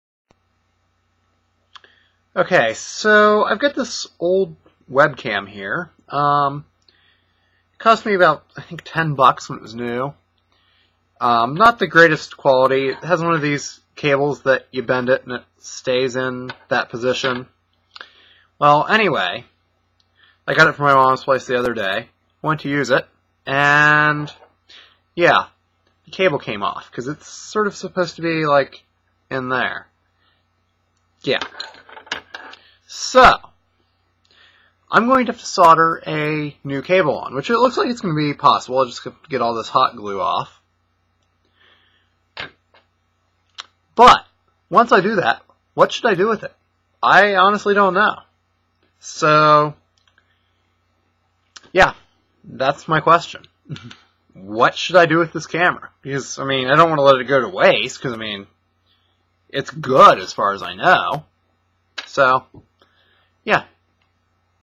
Crappy USB webcam... what to do with it?
So, I've got this cheap webcam with one of those cables that holds its position, and the cable separated. I'm gonna put a new cable on it, but what should I do with it?